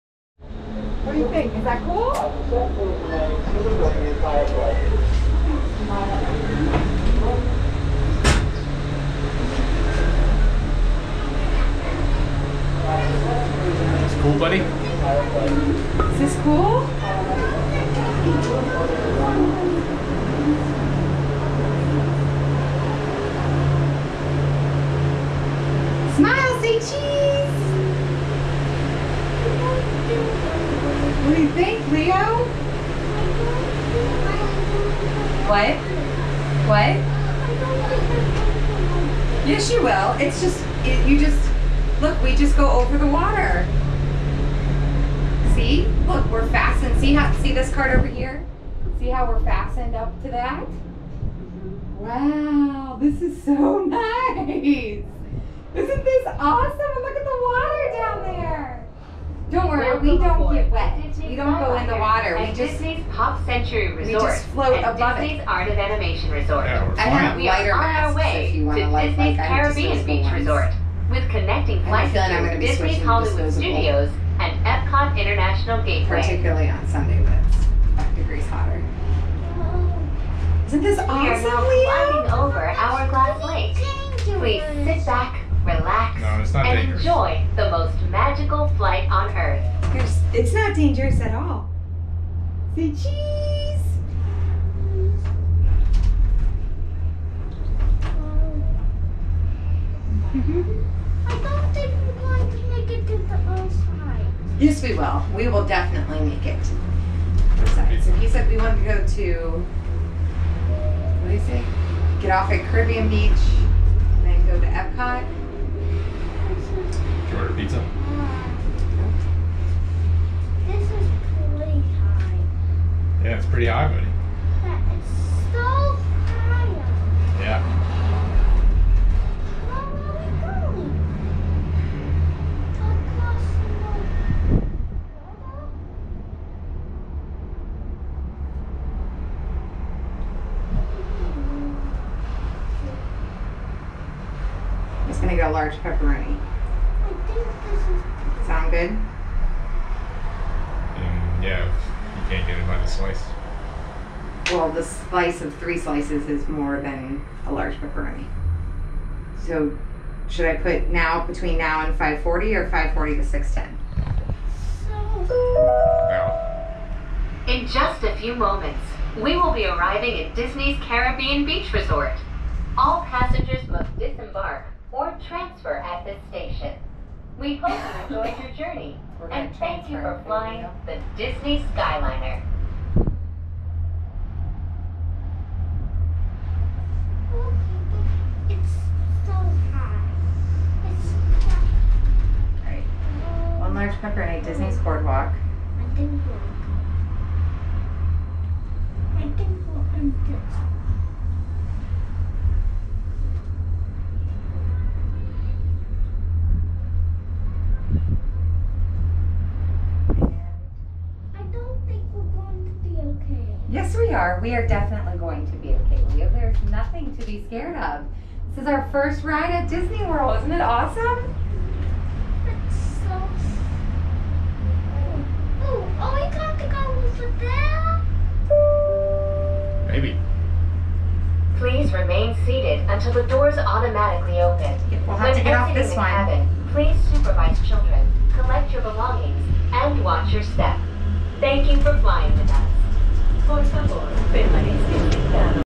VR/ 360 - Disney - May '21 - SkyRider
Leo, Kim and Matt riding the Skyrider from the hotel to the boardwalk.